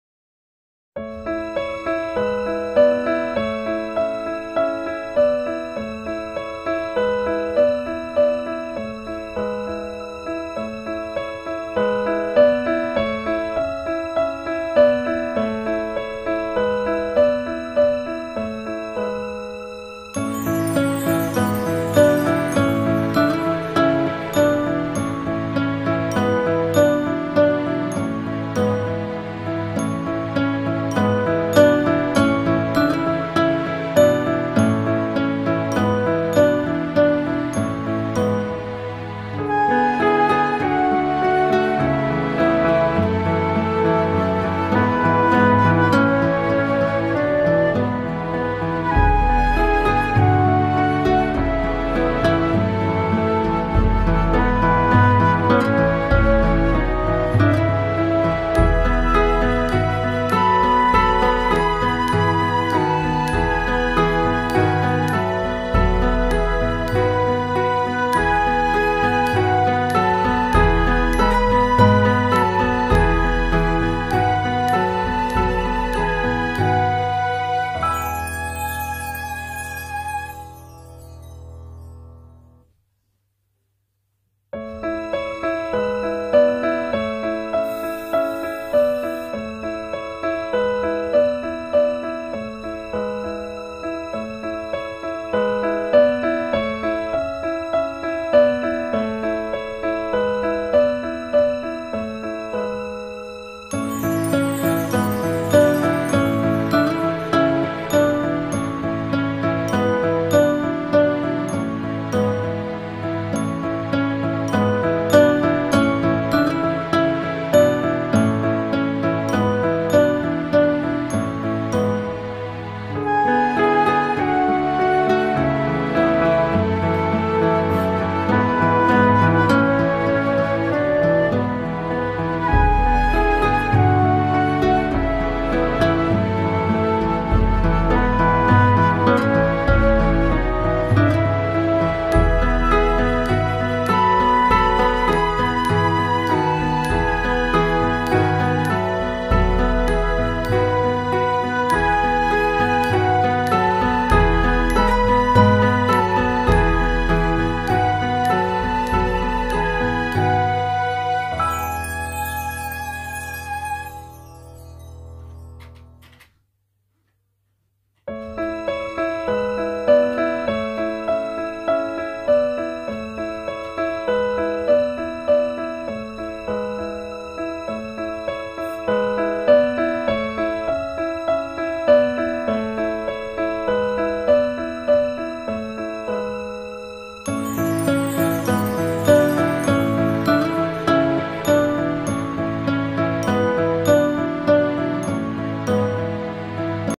Blessed Vessels Cleaning Services: Tutorial: Dusting and polishing
Blessed Vessels Cleaning Services:
High Dusting and Polishing in a room